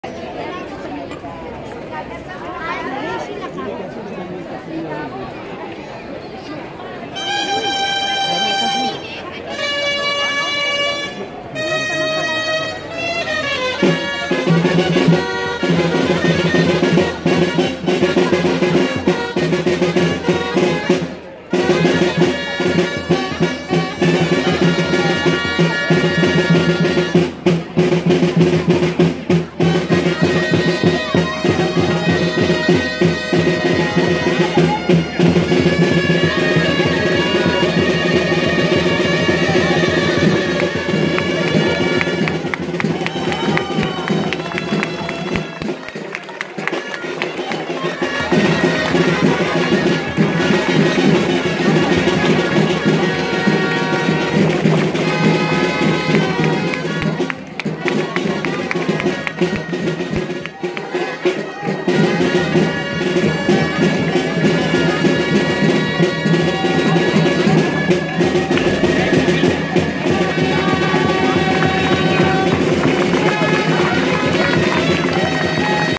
2p4 - Diada de Sant Roc a Rubí | 4K QHD
A la ronda d'inici , 2 pilars de 4 simultanis dels Castellers dels Castelldefels a la diada de Sant Roc a Rubí. Vam ser acompanyats pels castellers de Rubí i castellers de Sant Cugat.